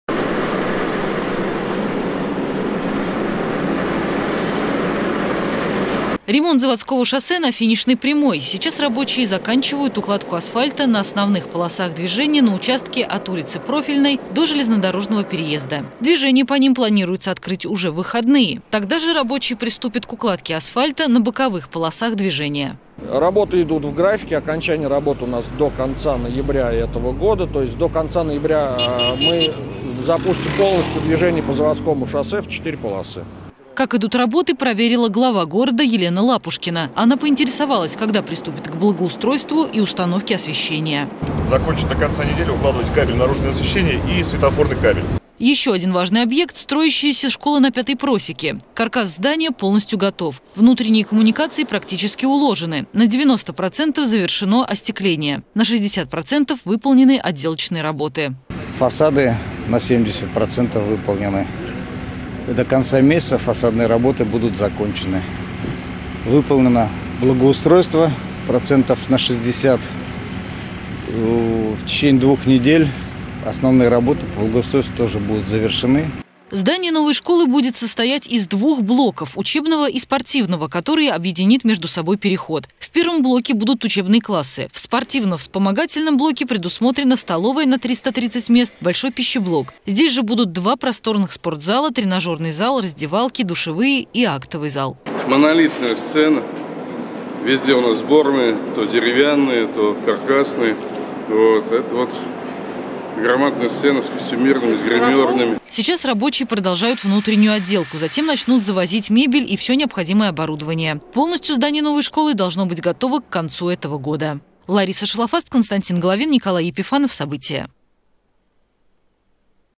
Движение по Заводскому шоссе в Самаре полностью откроют в ноябре
Движение по Заводскому шоссе в Самаре полностью откроют в ноябре. Напомним, масштабная  реконструкция началась в 2019 году. На участке от ул. 22 Партсъезда до Авроры проезжую часть расширяют до 4 полос, под которыми заранее прокладывают все необходимые городские коммуникации, строят тротуары, парковки, меняют остановочные павильоны, фонари. Как идет ремонт проверила глава города Елена Лапушкина. Успевает ли подрядчик в срок, выяснила и наша съемочная группа.